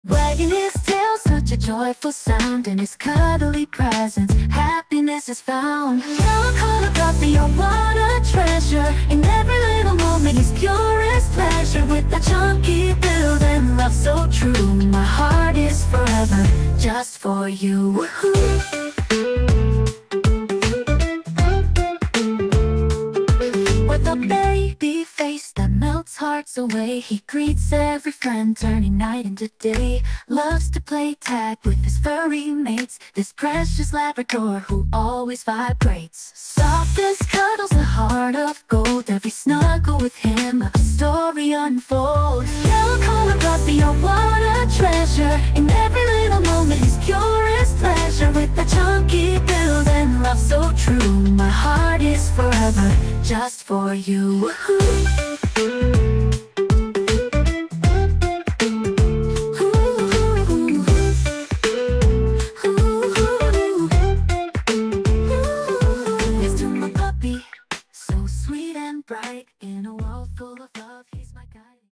Yellow collar boy
The largest and thickest build puppy in the litter is yellow collar.  He has a very sweet and gentle personality and reminds me so much of his dad Lumiere. ￼